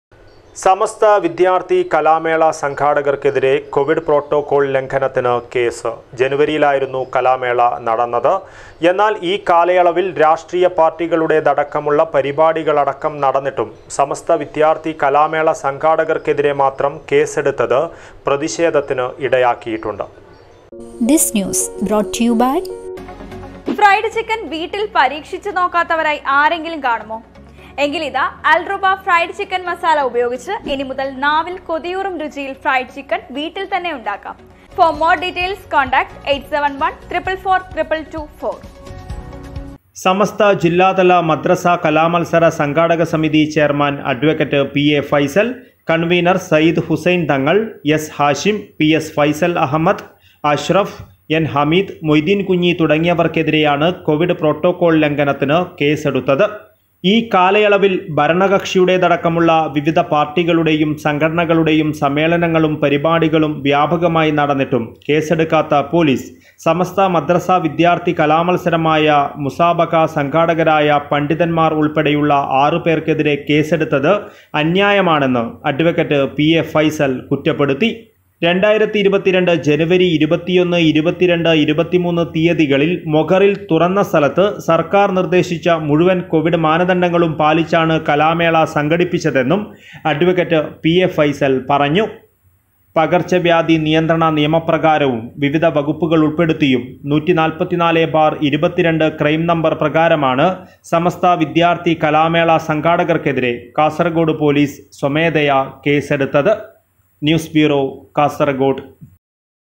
സമസ്ത വിദ്യാര്‍ത്ഥി കലാ മേള സംഘാടകര്‍ക്കെതിരെ മാത്രം കേസെടുത്തത് പ്രതിഷേധത്തിനിടയാക്കുന്നു
ഈ കാലയളവില്‍ രാഷ്ട്രീയ പാര്‍ട്ടികളുടേതടക്കമുള്ള പരിപാടികളടക്കം നടന്നിട്ടും, സമസ്ത വിദ്യാര്‍ത്ഥി കലാ മേള സംഘാടകര്‍ക്കെതിരെ മാത്രം കേസെടുത്തത് പ്രതിഷേധത്തിനിടയാക്കുന്നു.